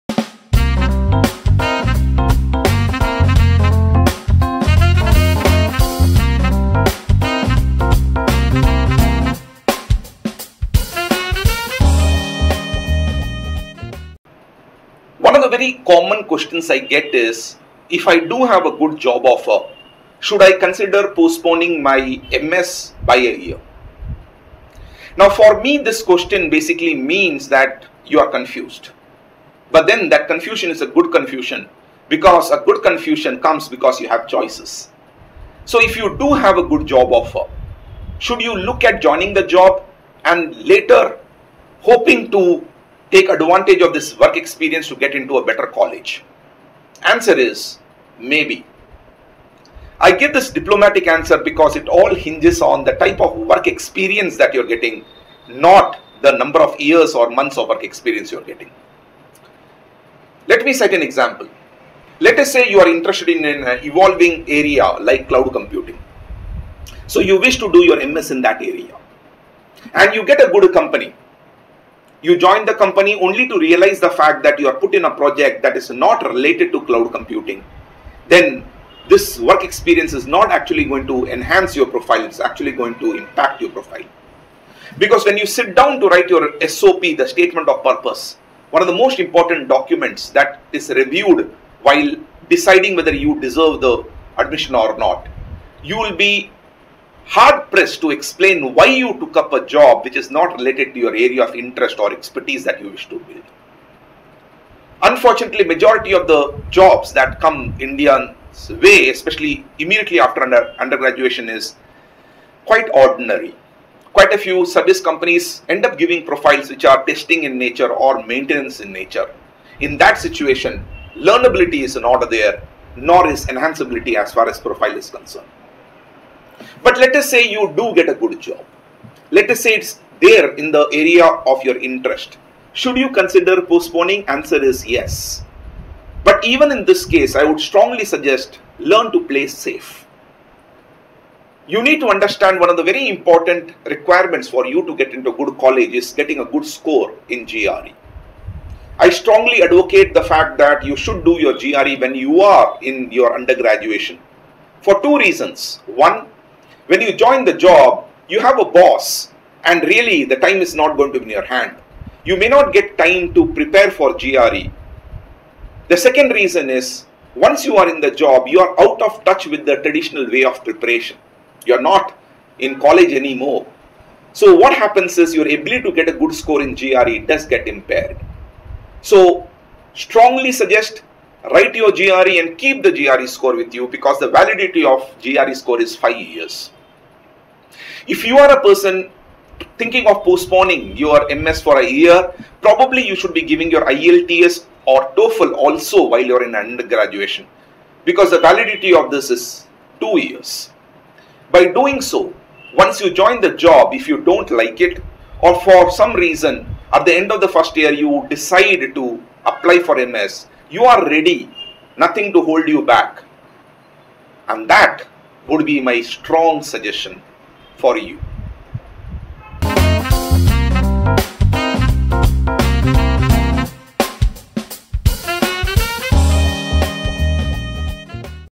Job or MS now?
One of the common questions I get is: Would a Job experience help me get into better schools for MS? In this video I answer that and also talk about an important strategy,